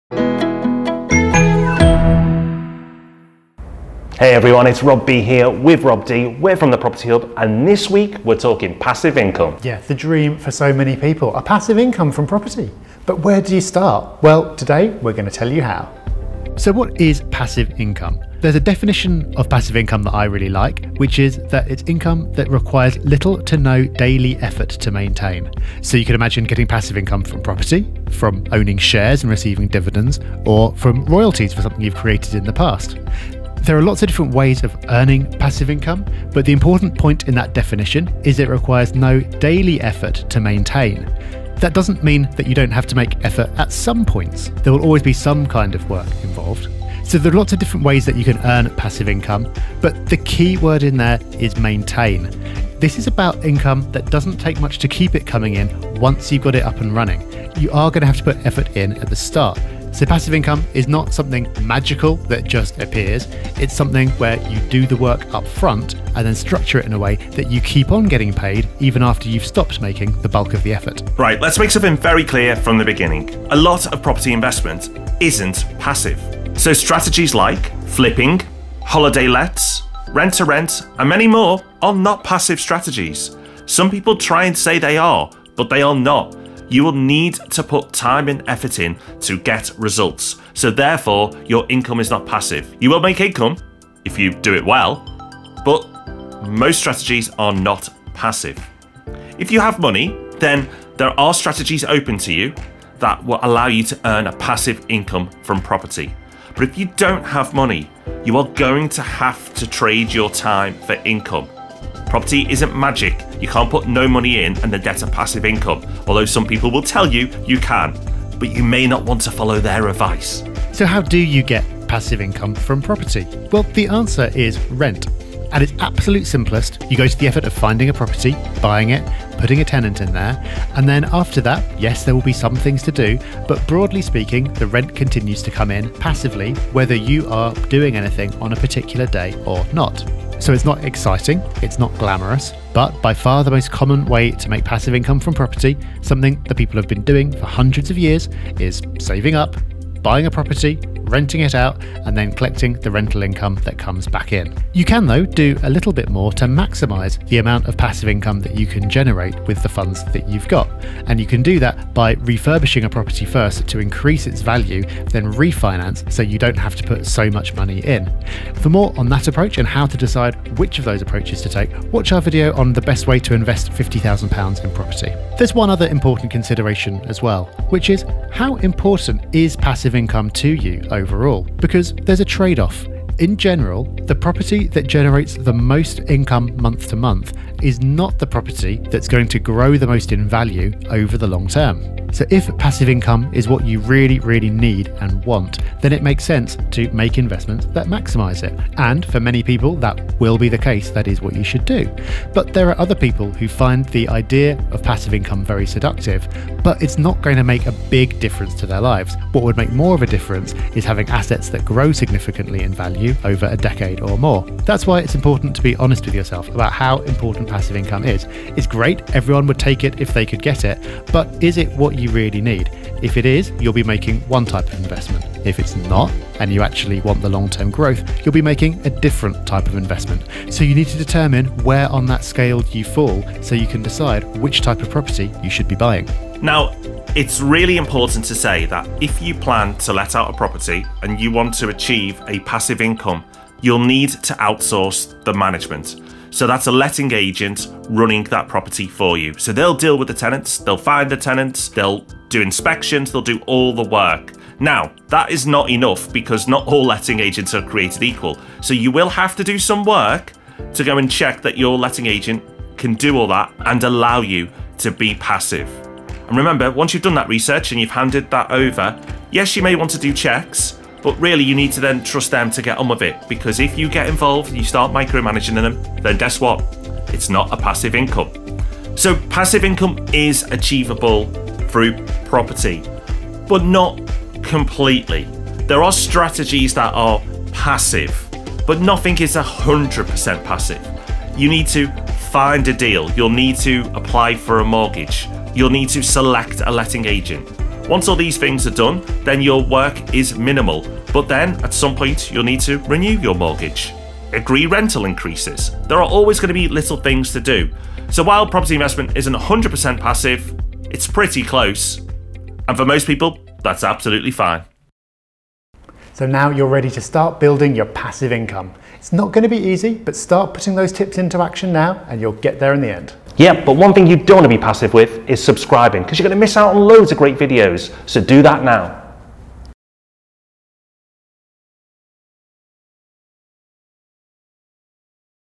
The truth about passive income
Income that requires little or no daily effort to maintain. Sounds great, right? It might come as a shock to you, but not a lot of property investment strategies are passive.

Nor can you run with a 'no money down deal' and expect to get a passive income - despite what many might tell you.

If you're here to find out how to achieve a passive income from property properly. watch this video now.